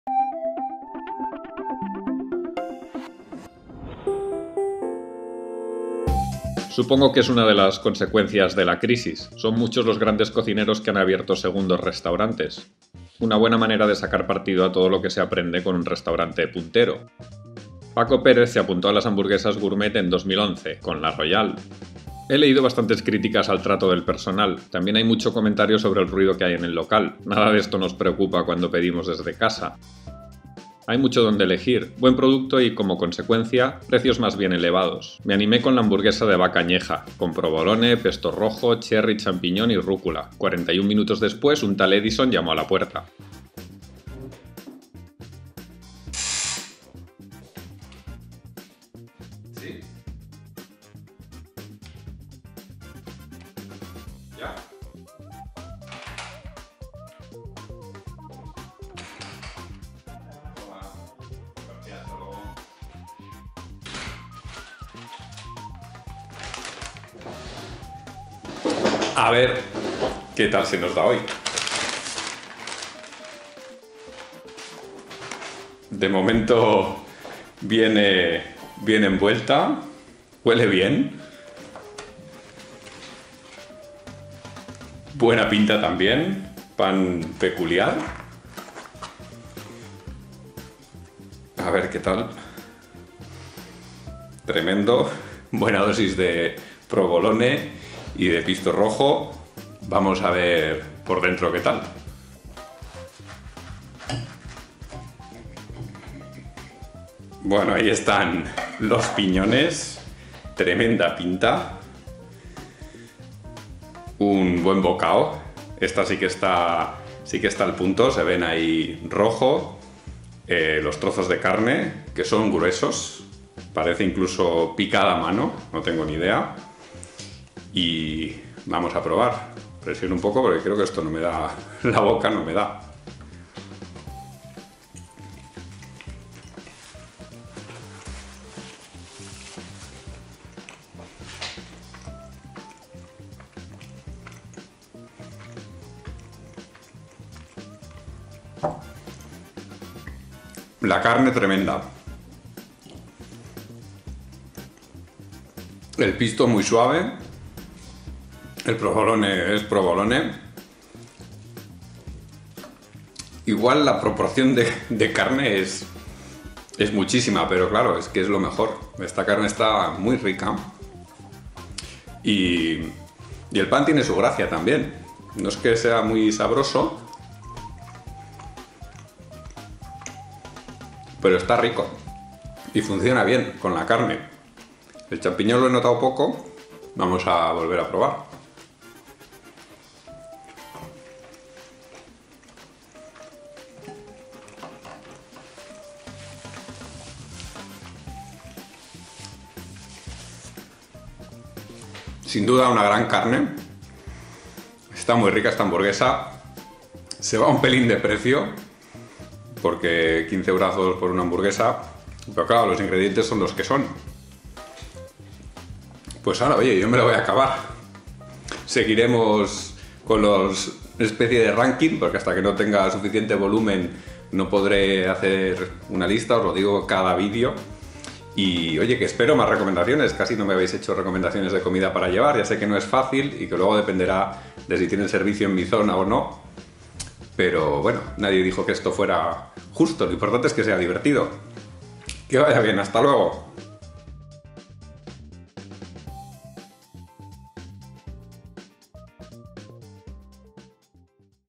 TAKE-OUT BARCELONA #3 LA ROYALE
Seguimos buscando buena comida a domicilio. Hoy, la super hamburguesa de La Royal de Paco Pérez.

La Royale

Pedido mínimo 12,00 €
Entrega 3,99 €
Vaca añeja 12,90 €